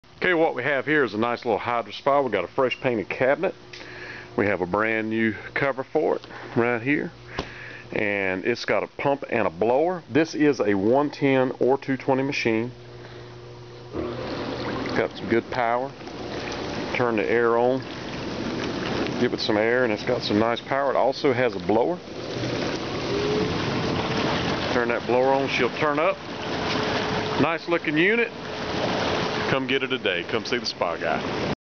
Used Hydro Spa New Cover, Fresh Painted Sides, 24 Total Jets, 115V plug and Play
Nice 7X7 Hydrospa Has Hydroquip Electronics, Fresh Painted Side Panels, Has a New Cover it is 115V Plug and Play. or call Billy " The Spa Guy"